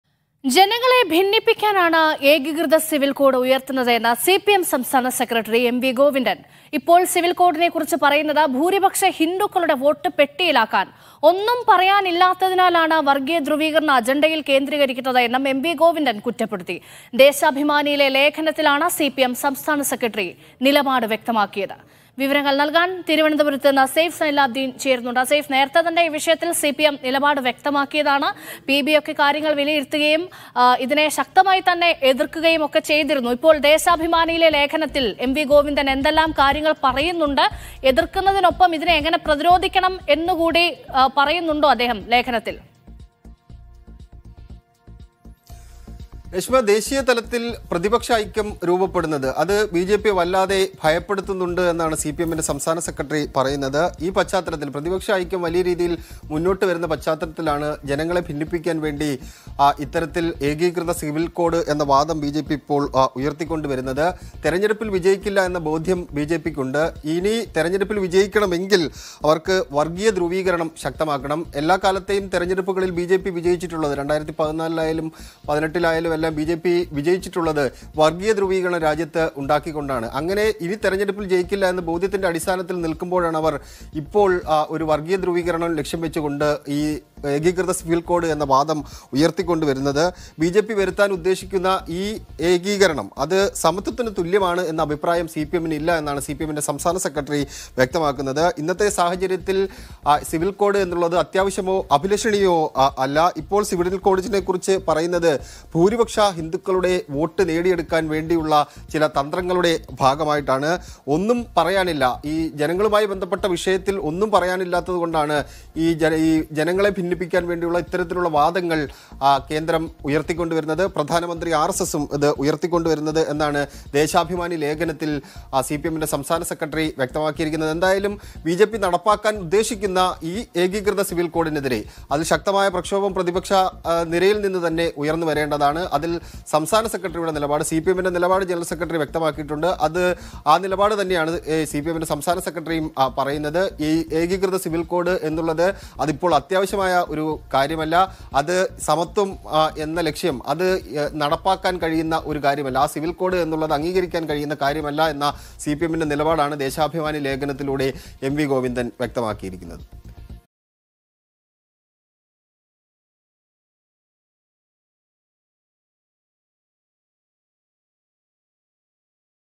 സിവിൽ കോഡ് ജനങ്ങളെ ഭിന്നിപ്പിക്കാൻ, ഹിന്ദു വോട്ട് ലക്ഷ്യം: എം.വി ഗോവിന്ദൻ
Civil Code To Divide People, Target Hindu Vote: MV Govindan